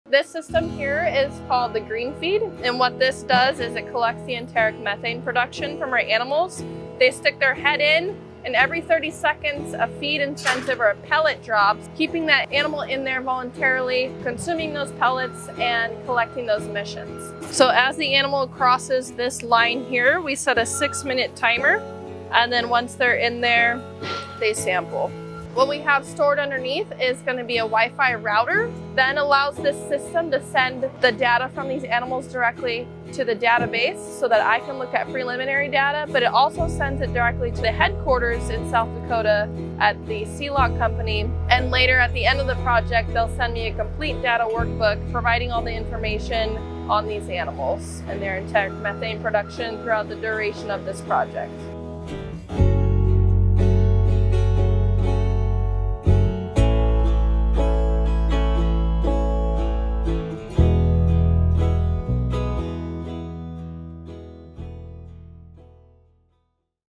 GreenFeed Measures Methane Emissions from Cattle Burps
At UC Davis, graduate students in the animal science department at the College of Agricultural and Environmental Sciences are using C-Lock's GreenFeed system to measure enteric methane emissions from cattle. The system allows for an accurate measurement of methane emissions produced by ruminant belches during nutrition-based research trials using different feed additives like tannins.

Learn more about the CLEAR Center and what we do